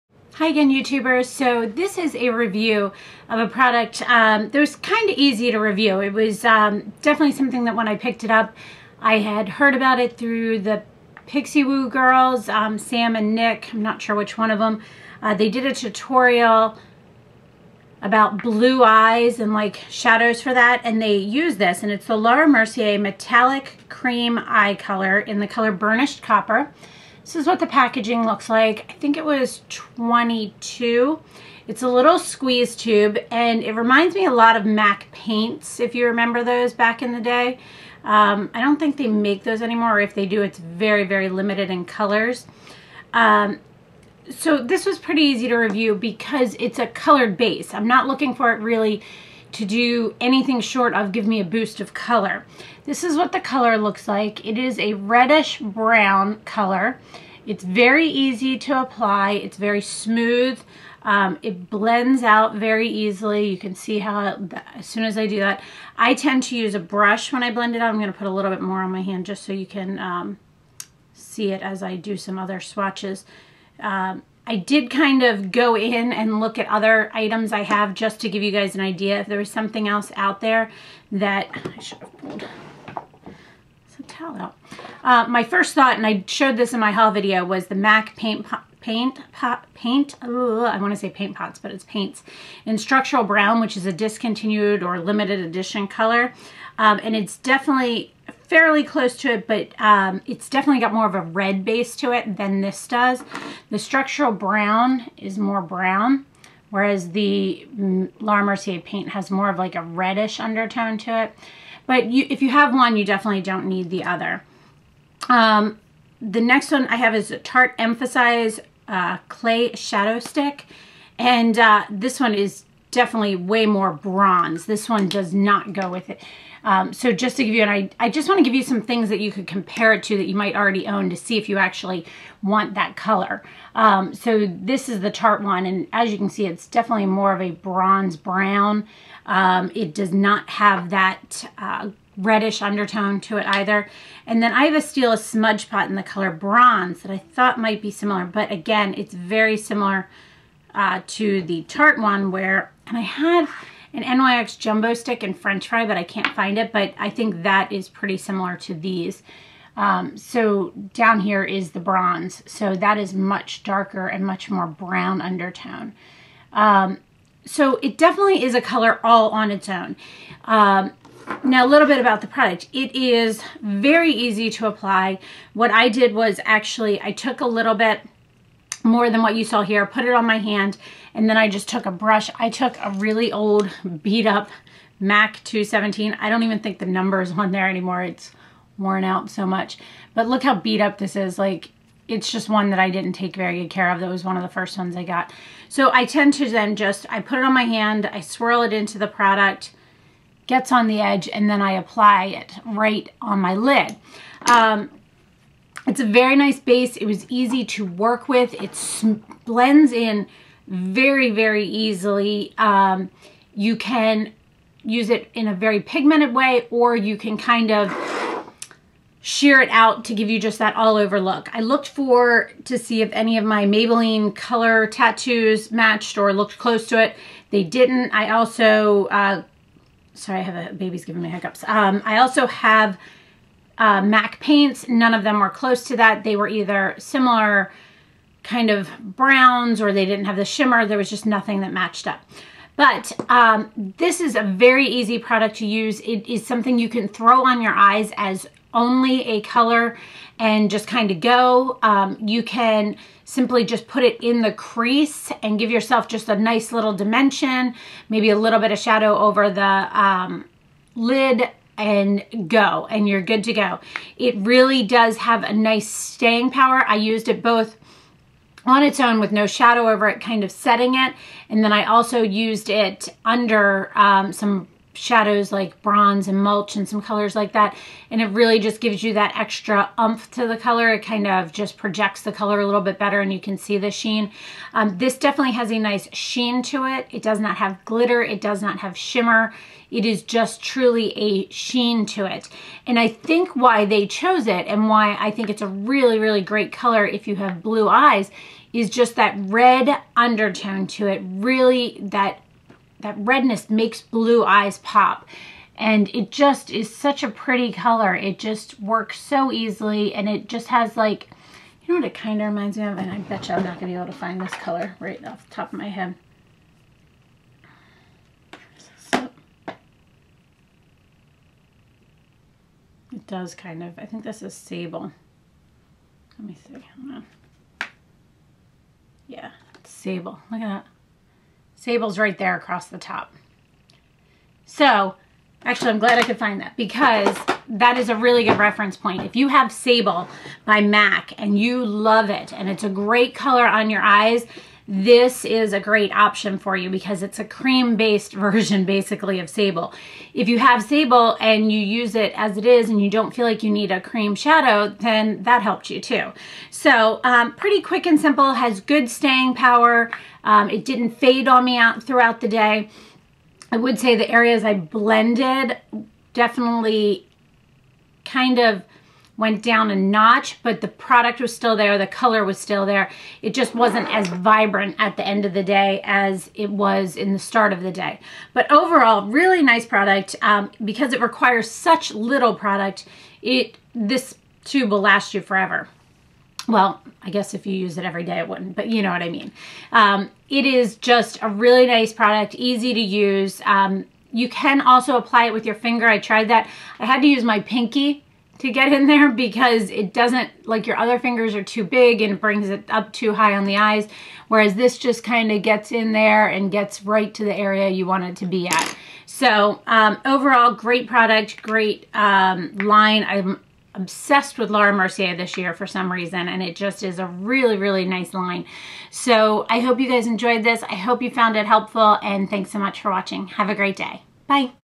Review Laura Mercier Cream Shadow
I am wearing:
LM Radiance, Hourglass veil, LM Mineral powder (mix of real sand and classic beige) LG Bronze and Brughten, Youngblood Rouge, Dior Amber Diamond
Eyes- MAC Yogurt, embark, Novalash liner, MAC Charcoal Brown on brows
Lips- MAC Whirll, MAC hot Gossip, MUFE S2 gloss
Nails- OPI for Sephora "It's somewhere in my purse"- color name

*I purchased the items in the video